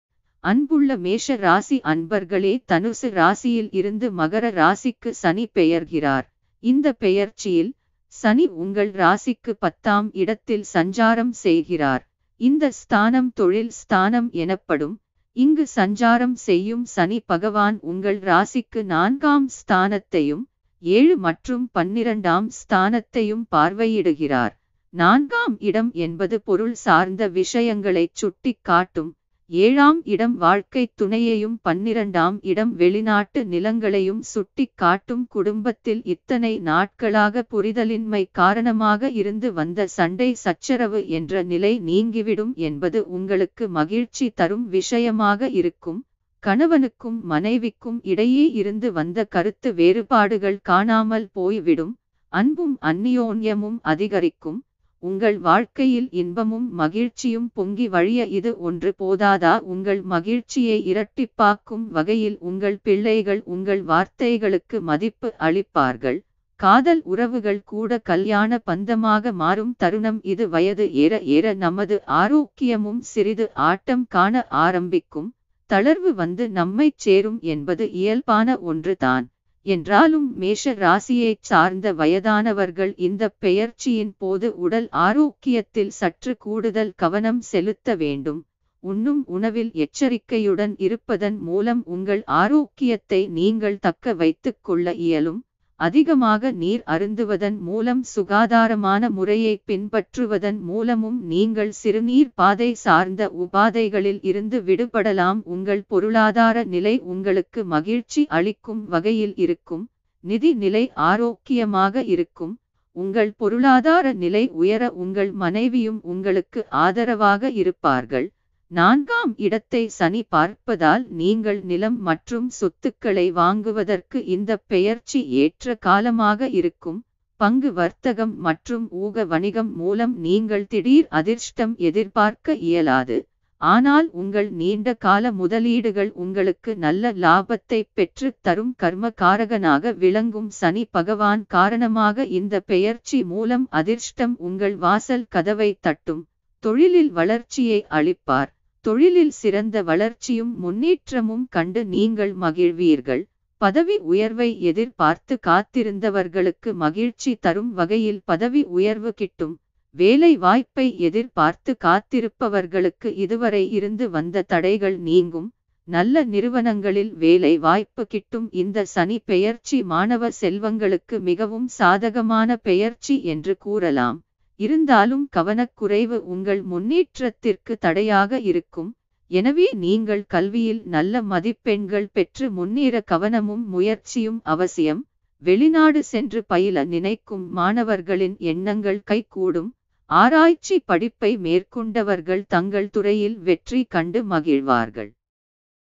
மேஷம் ராசி சனி பெயர்ச்சி பலன்கள் 2020-2023 | Mesha Rasi Sani Peyarchi Palangal
சனி பெயர்ச்சி 2020 எப்போது?

ஜனவரி 24 2019 சனி பெயர்ச்சி
2020 ஆம் ஆண்டு ஜனவரி 24 ஆம் தேதி சனி பகவான் தனுசு ராசியிலிருந்து மகர ராசிக்குள் பிரவேசிக்கிறார்.